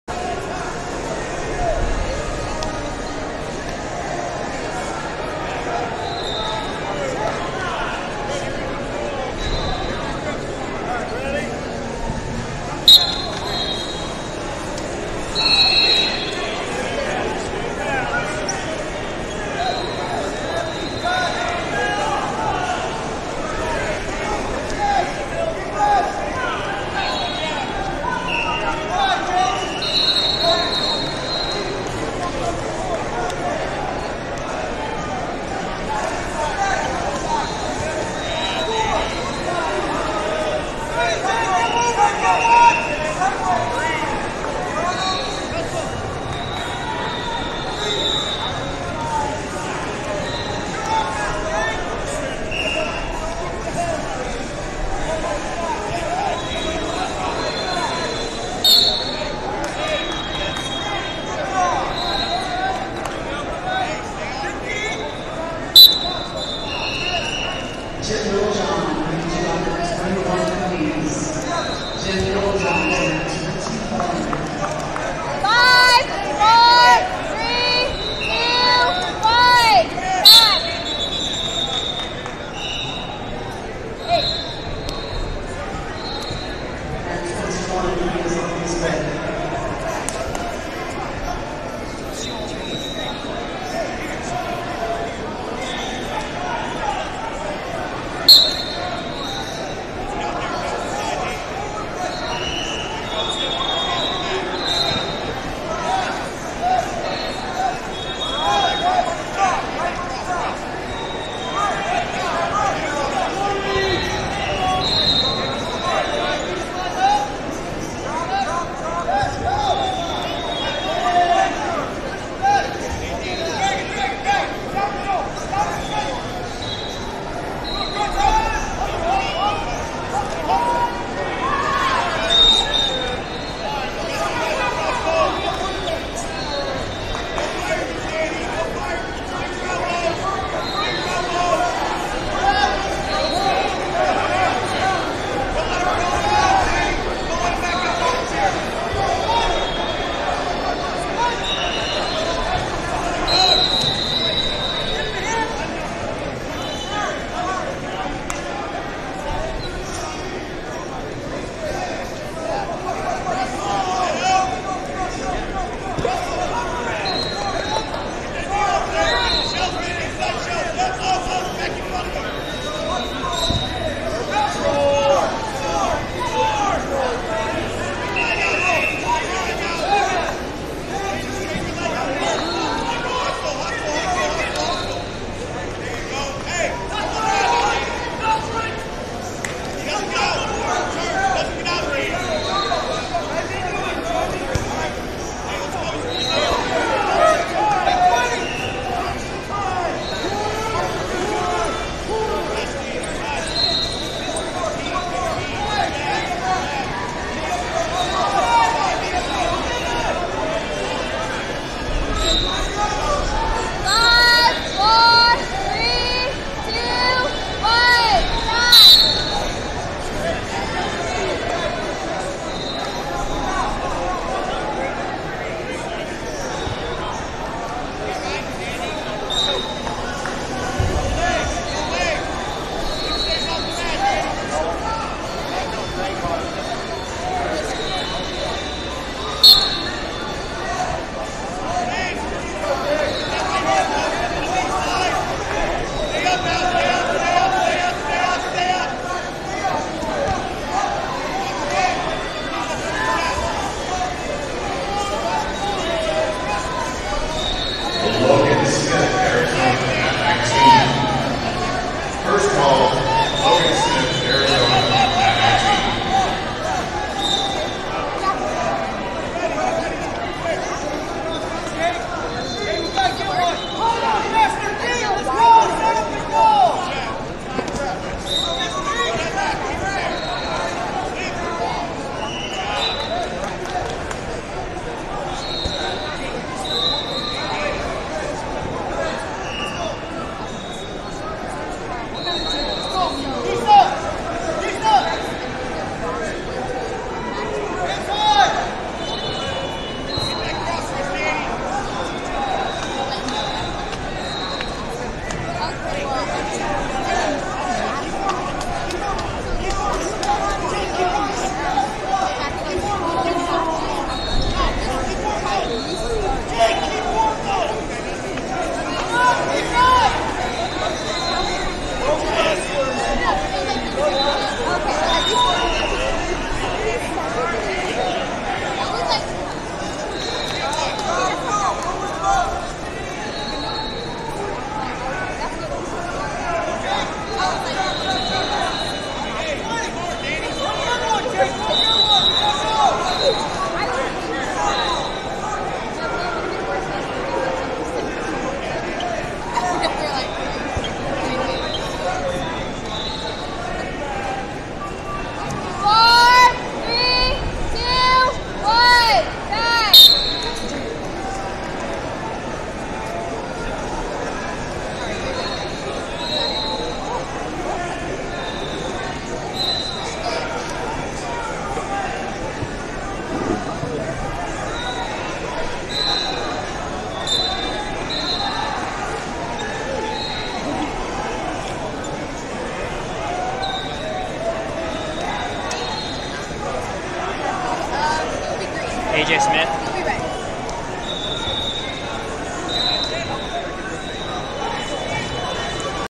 120 Lbs Cons. Round 3 - Jake Castagneto, ID Vs Daniel Aranda, IL 896a
Flosports-partner-access/2022 Brian Keck Memorial Preseason Nationals-Recruiting Showcase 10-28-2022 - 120 Lbs Cons. Round 3 - Jake Castagneto, ID Vs Daniel Aranda, IL 896a